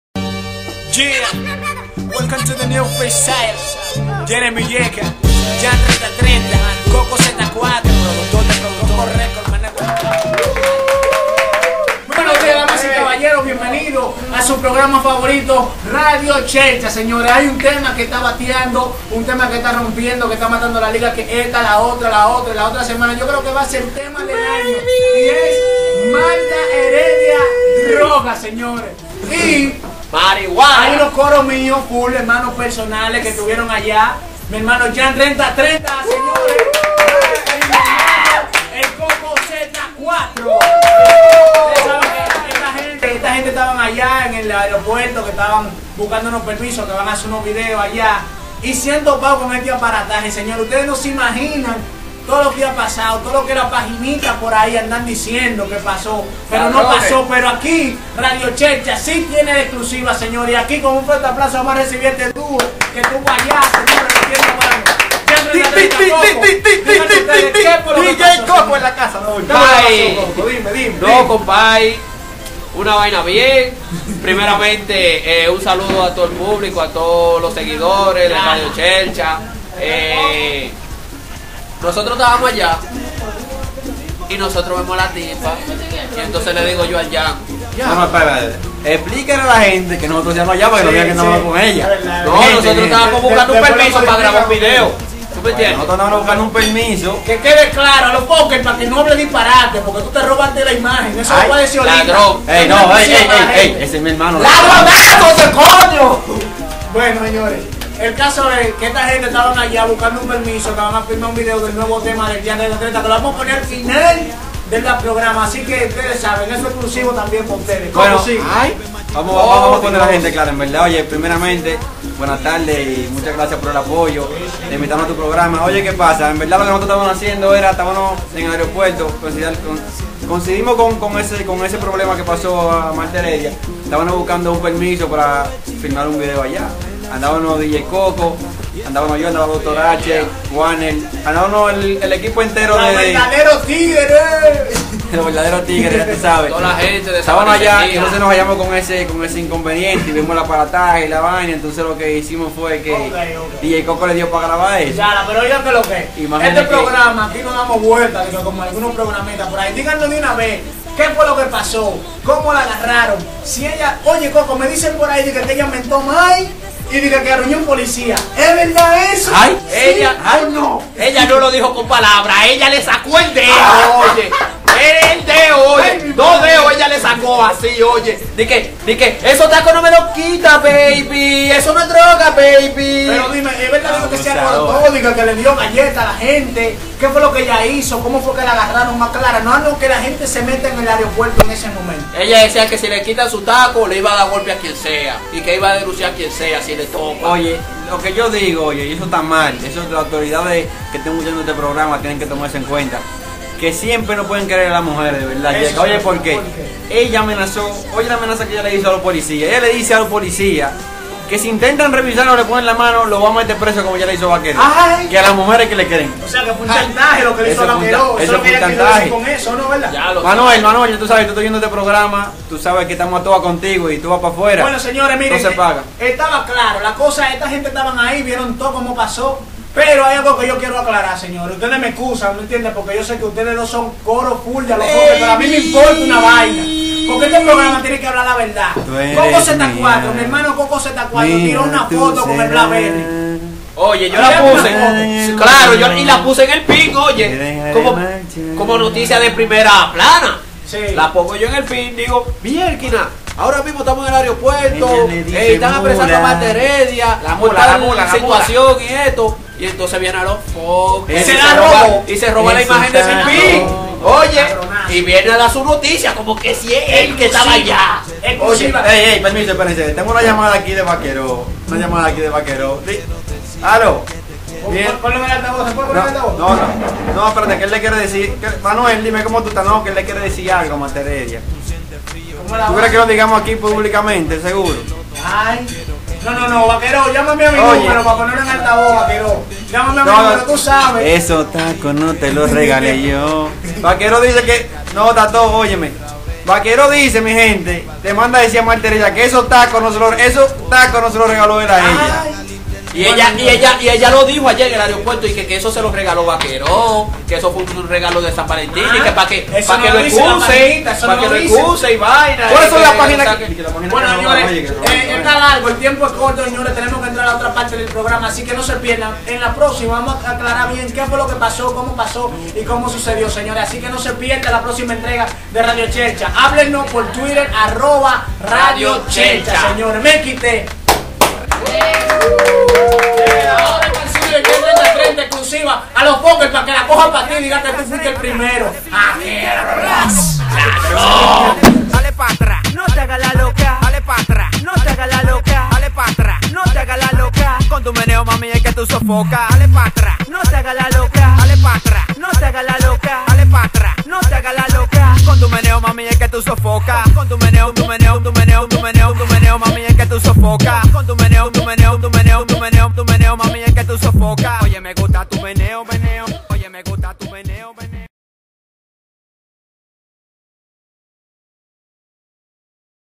La verdad de Martha Heredia - Radio chelcha
@radiochelcha programa de radio popular que entrevisto a dos cantates que presenciaron el hecho...